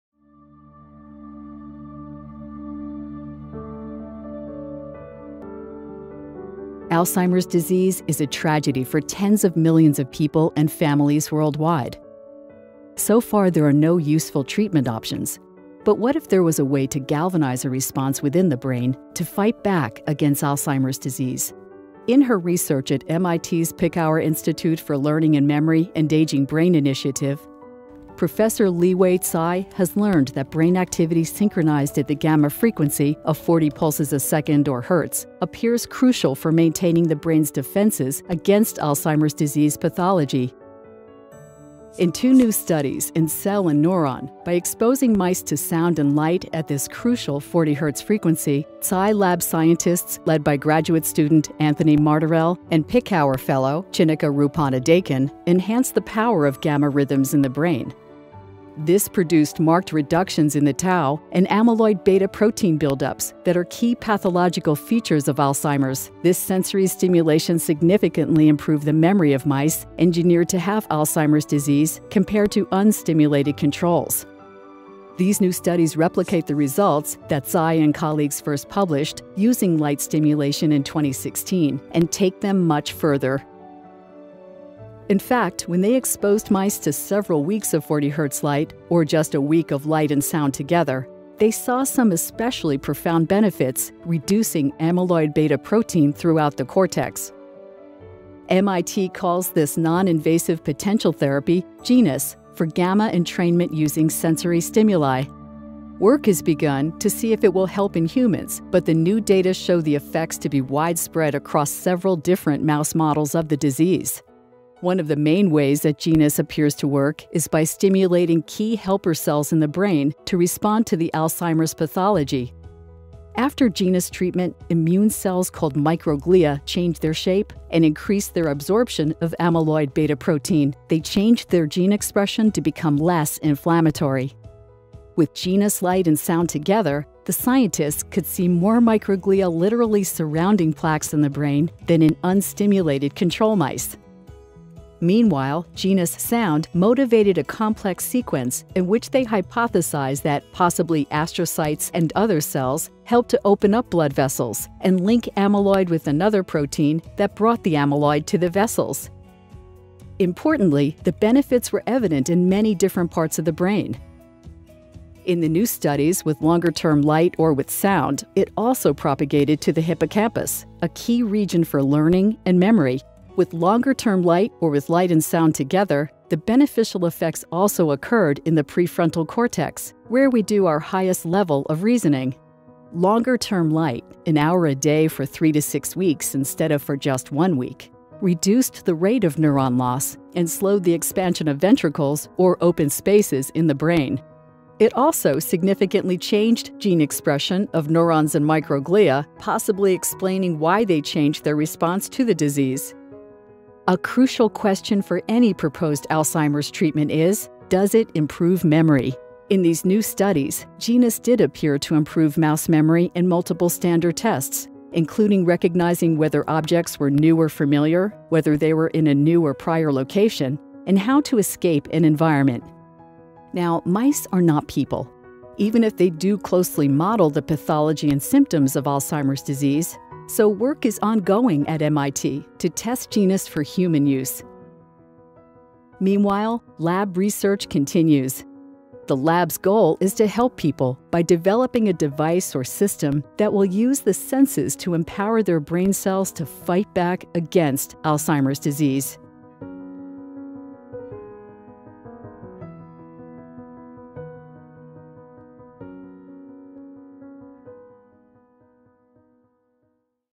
MIT Scientists Reveal Brain Rhythm Role in Alzheimer’s Research
MIT neuroscientists in the lab of Professor Li-Huei Tsai at The Picower Institute for Learning and Memory are studying whether sensory stimulation — light and/or sound — can reduce Alzheimer's disease pathology and improve memory by increasing the power of gamma rhythms in the brain. Two papers in 2019 reported encouraging results in multiple mouse models of the disease. Research is getting underway with human volunteers.

Credit: The Picower Institute for Learning and Memory